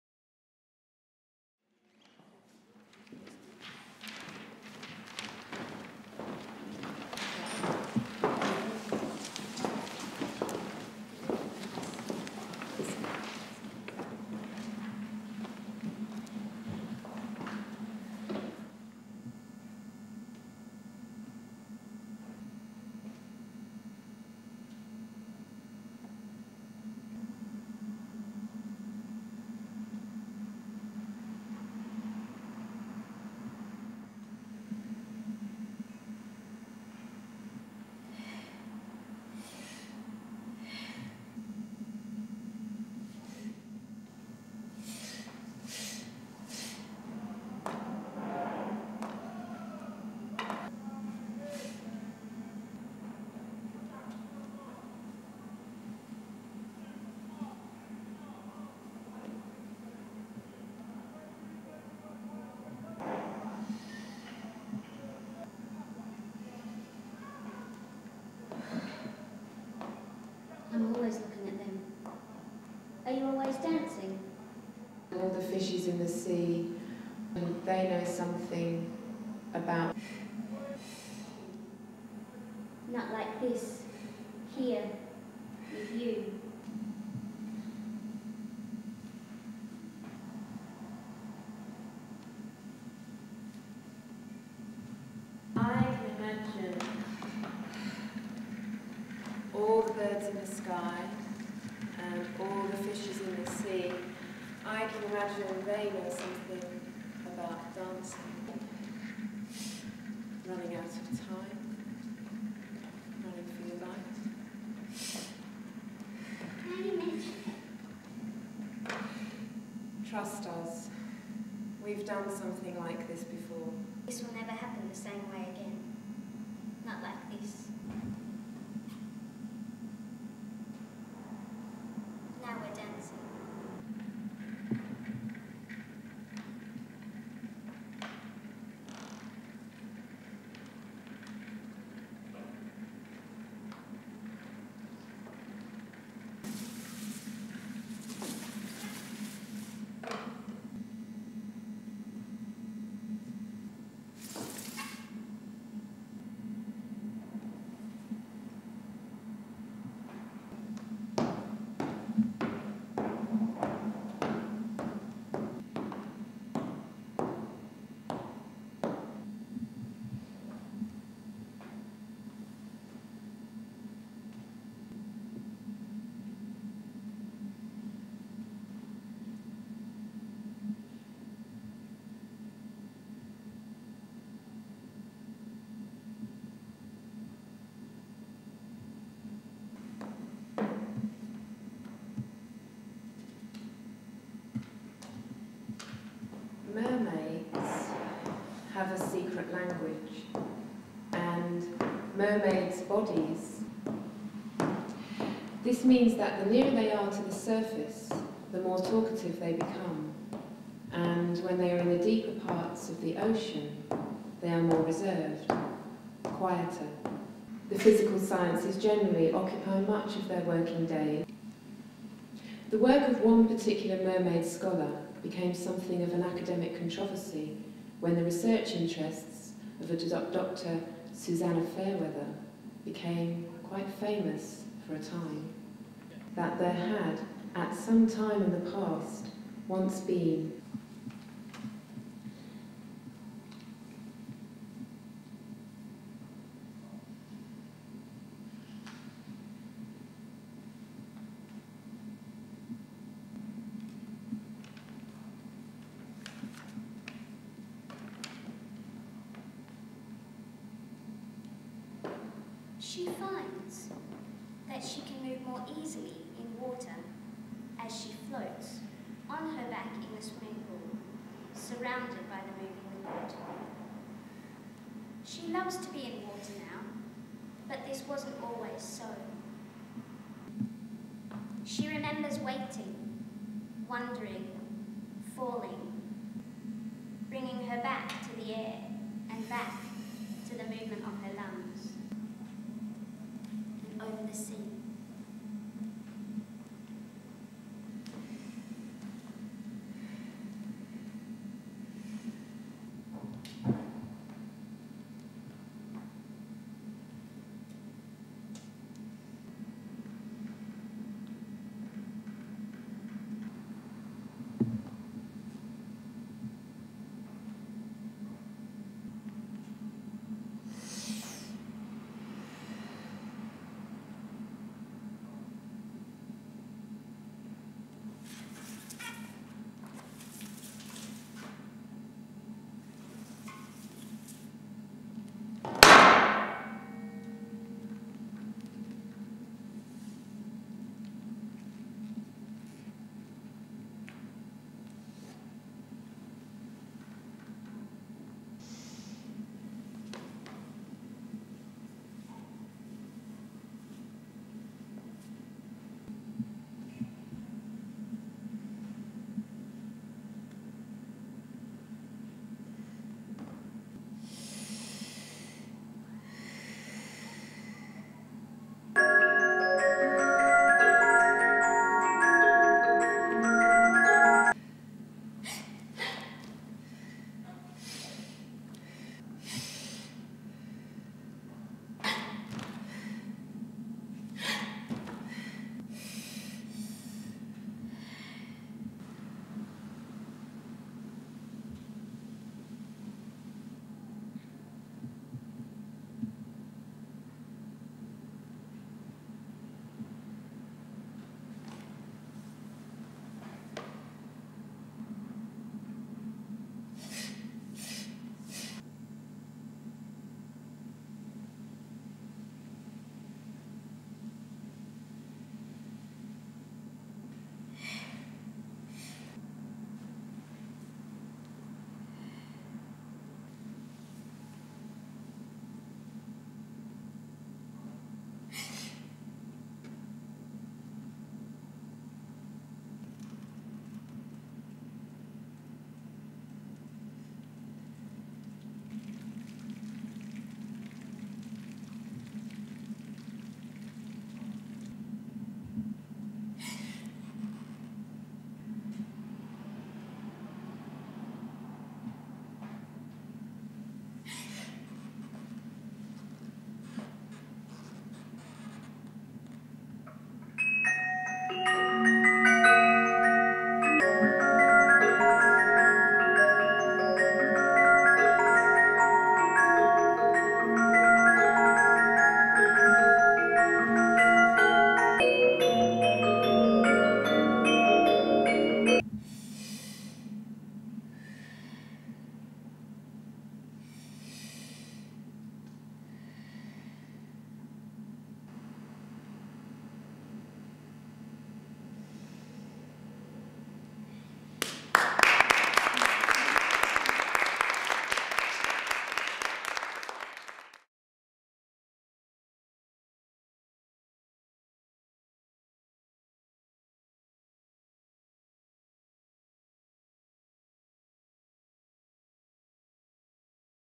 This Two
This Two is a highly unusual and gently provocative duet performance. A delicate contemplation of our views of the dancing and writing body, offering the audience questions about particular and personalised ways of moving and being moved. Choreographed and performed by Caroline Bowditch and Fiona Wright. Filmed and edited by Becky Edmunds